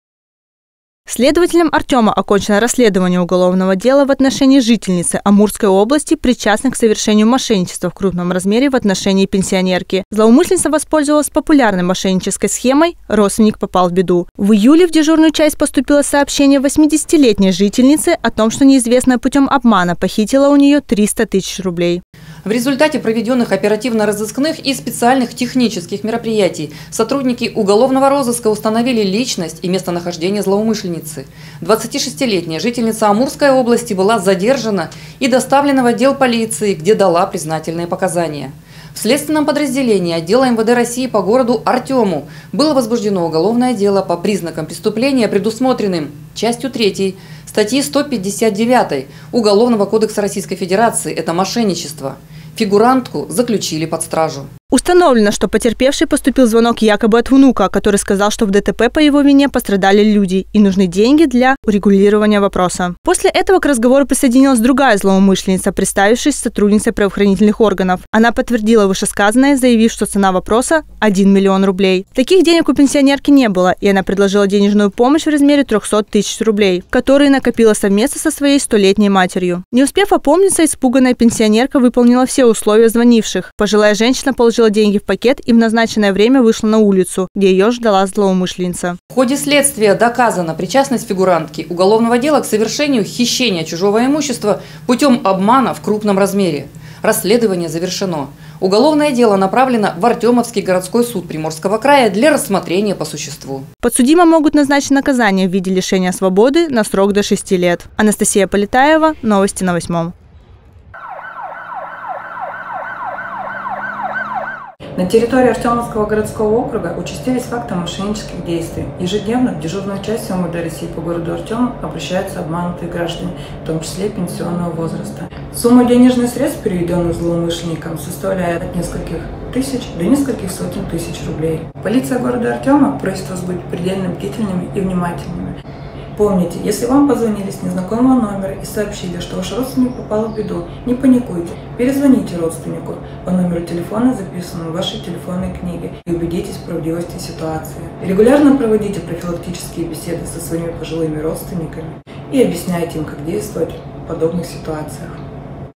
В Приморье будут судить жительницу Амурской области, завладевшую деньгами пенсионерки.
Следователем следственного подразделения отдела МВД России по городу Артему окончено расследование уголовного дела в отношении жительницы Амурской области, причастной к совершению мошенничества в крупном размере в отношении пенсионерки. Злоумышленница воспользовалась популярной мошеннической схемой «Родственник попал в беду.